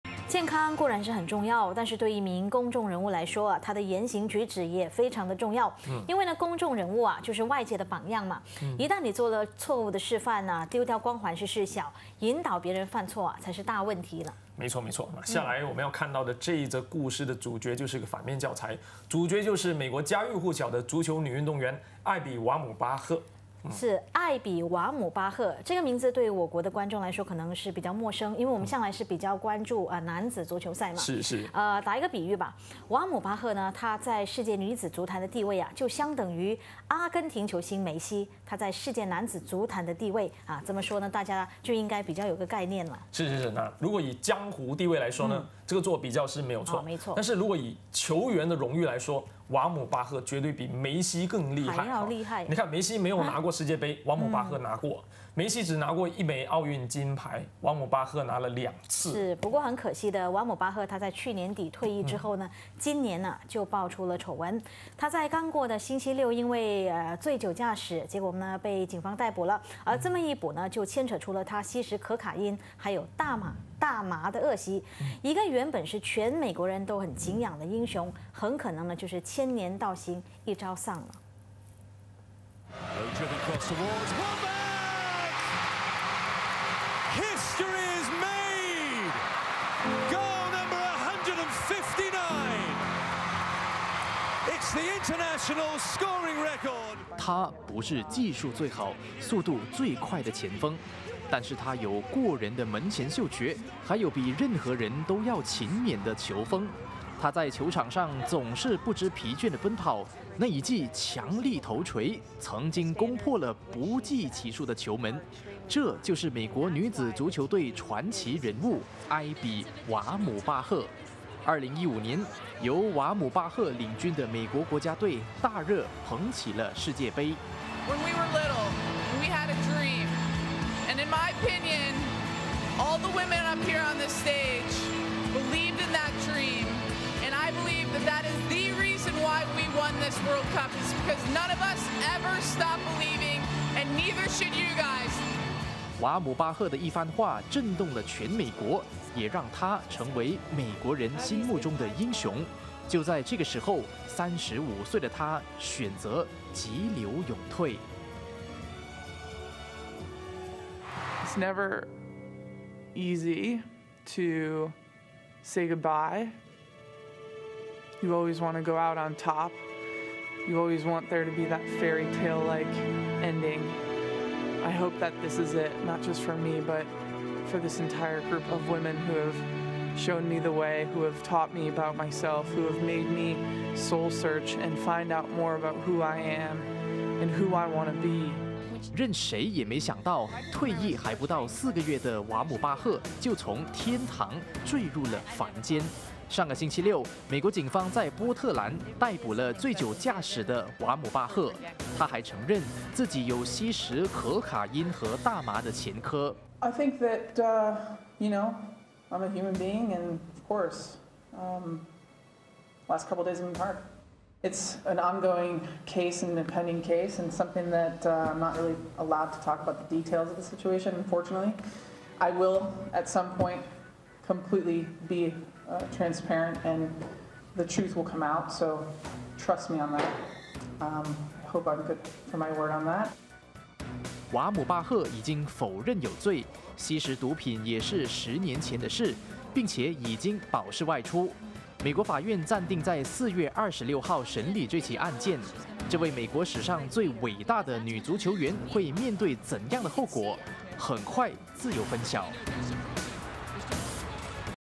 美国女足传奇酒驾被捕 千年道行一朝丧？ [20160408]
美国家喻户晓的足球女运动员艾比瓦姆巴赫，对我国观众来说是个陌生的名字。因为我们向来都比较关注男子足球赛。打个比喻好了，瓦姆巴赫在世界女子足坛的地位相等于阿根廷球星梅西在世界男子足坛的地位。这么说，大家应该比较有概念了吧！

如果以江湖地位来说，这么做比较是没错。但是以球员荣誉来说，瓦姆巴赫绝对比梅西更强！梅西没拿过世界杯，瓦姆巴赫拿过！梅西只拿过一枚奥运金牌，瓦姆巴赫拿了两次！

不过，很可惜，瓦姆巴赫去年底退役后，今年就爆出丑闻。她在过去的星期六因为醉酒驾驶被警方逮捕。这么一捕，还牵扯出她吸食可卡因和大麻的恶习。一个原本是全美国人景仰的英雄，很可能千年道行一朝丧。